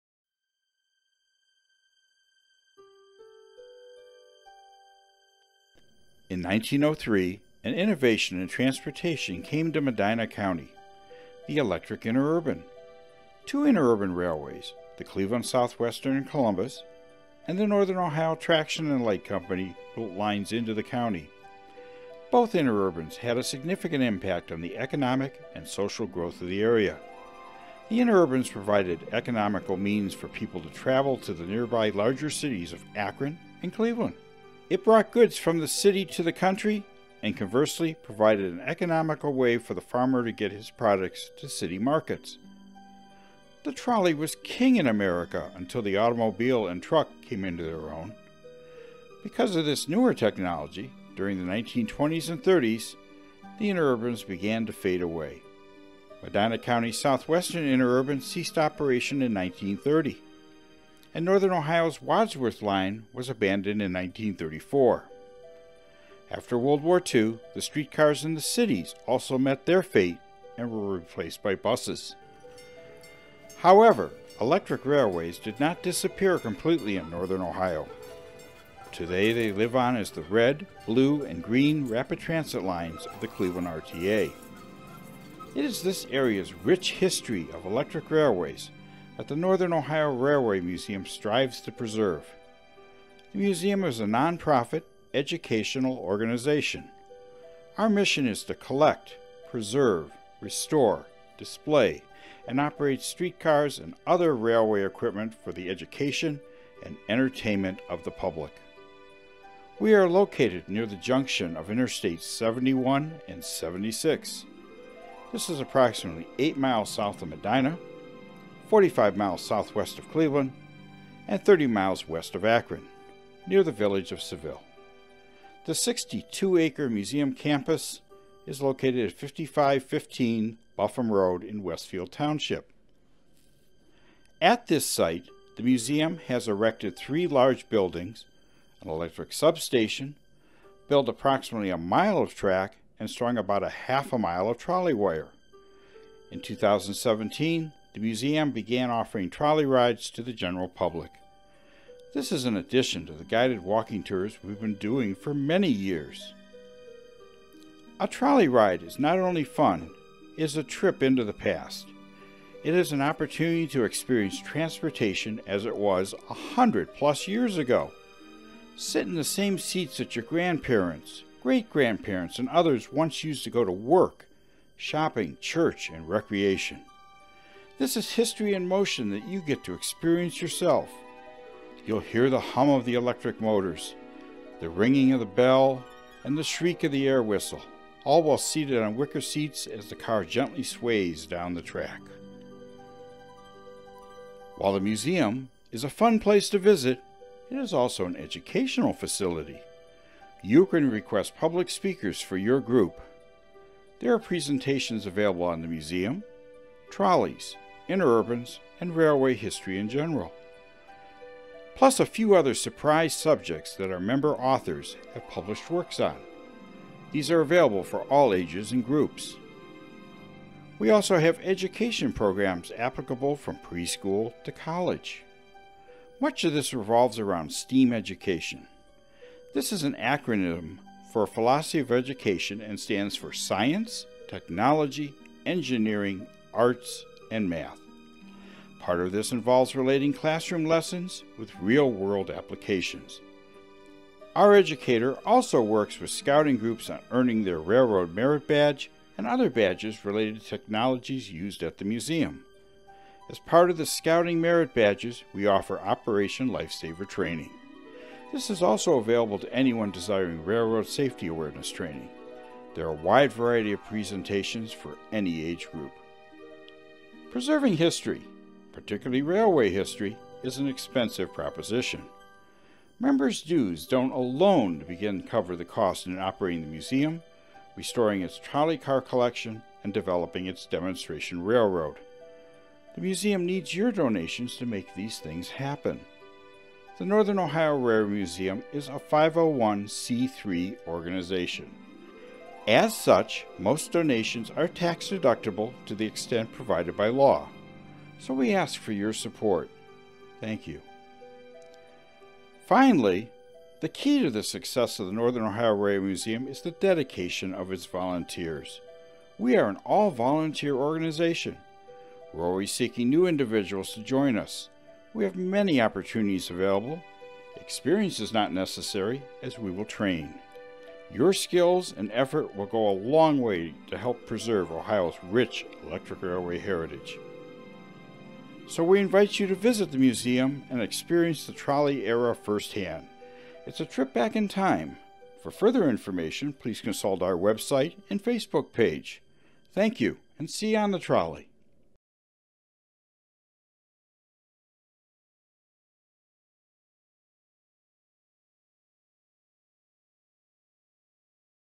Northern Ohio Railway Museum
Discover the Northern Ohio Railway Museum’s site, collections, and educational opportunities.  for more information on the museum and ways to support it.